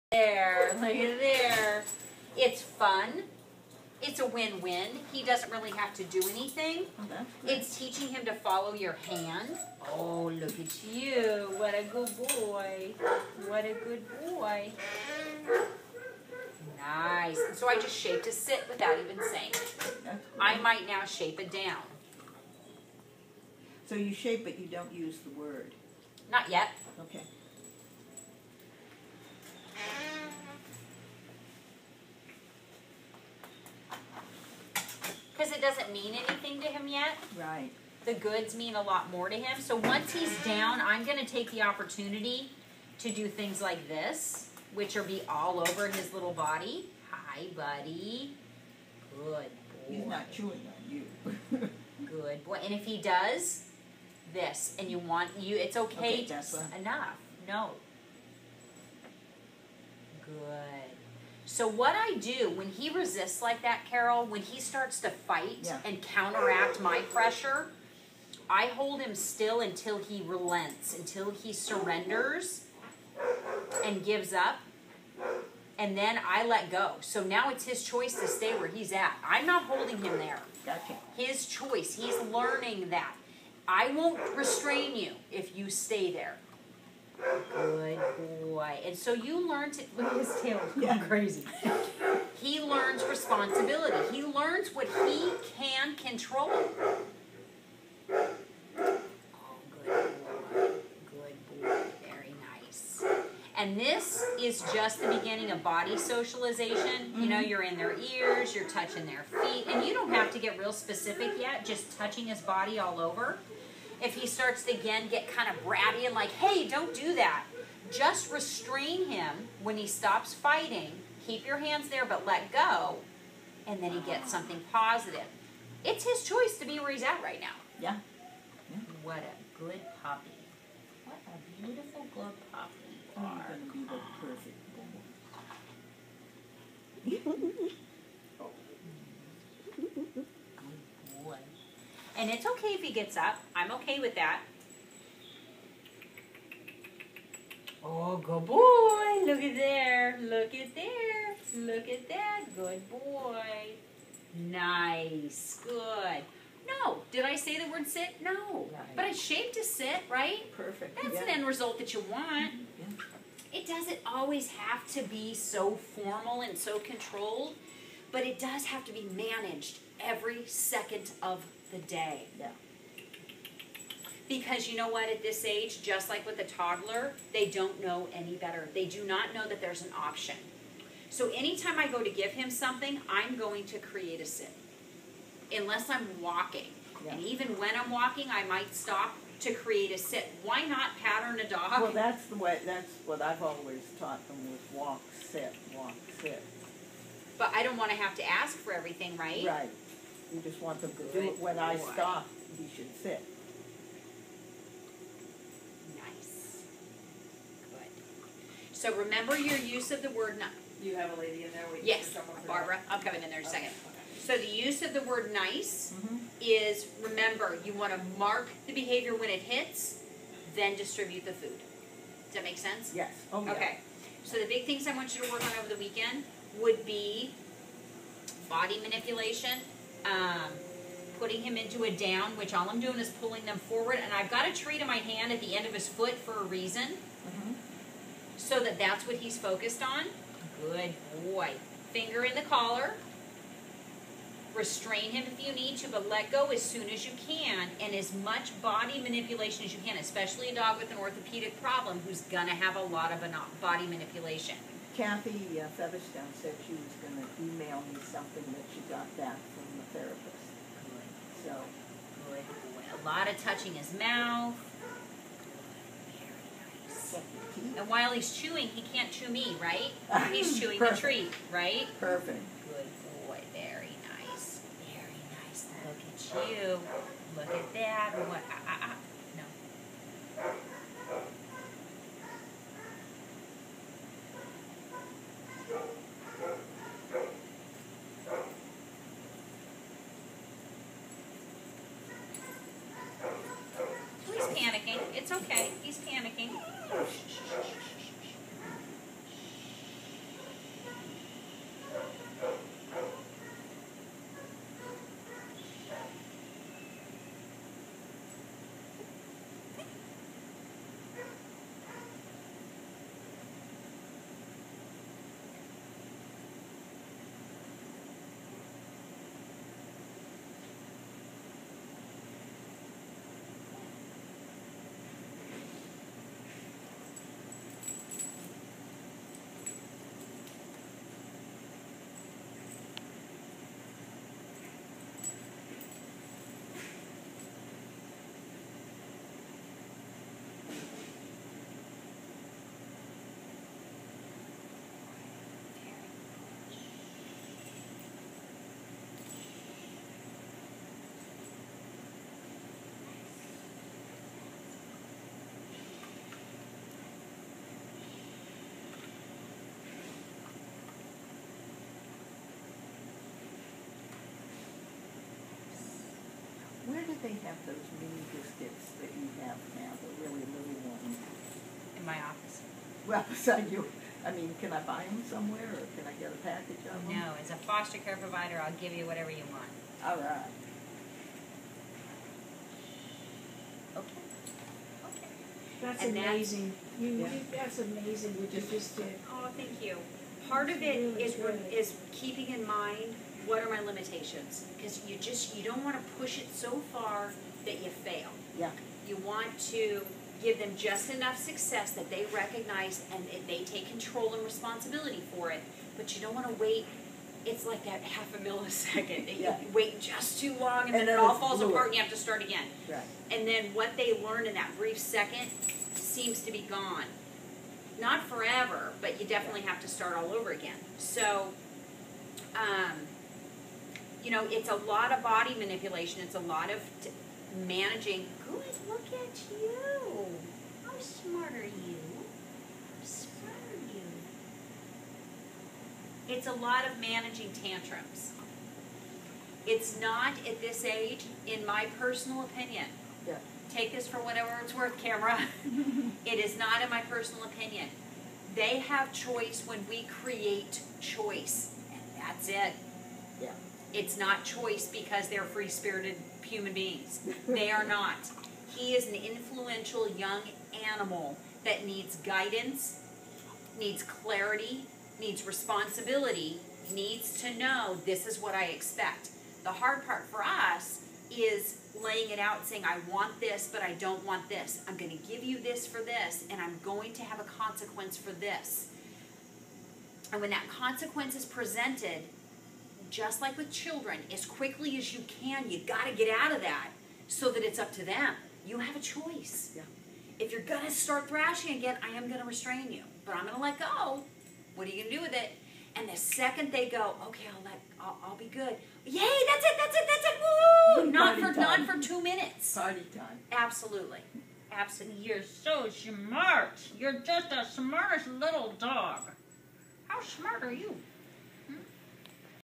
Teri teaches Wheeler to chill
Here's dog trainer, Teri Balestrieri, showing Wheeler the pup how to be a good boy and settle with calm body handling.  She is also instructing his volunteer foster care provider how to use positive reinforcement techniques for shaping desirable behavior